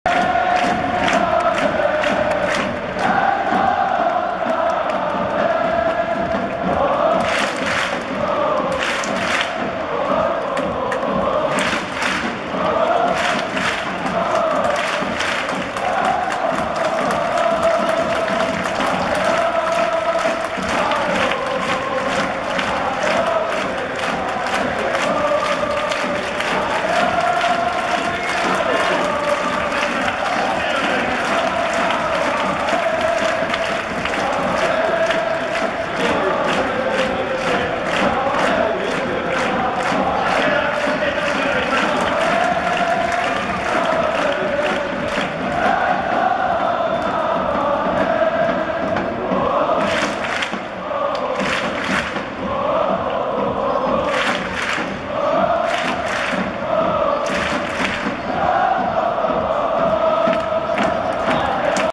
選手交代 89分渡邊 凌磨 → 中島 翔哉、89分サミュエル グスタフソン → 武田 英寿 2024明治安田J1リーグ 第36節 VS サンフレッチェ広島戦 ＠埼玉スタジアム2002
メインロアー北側で撮影。
画質等良くないので雰囲気だけ味わって下さい。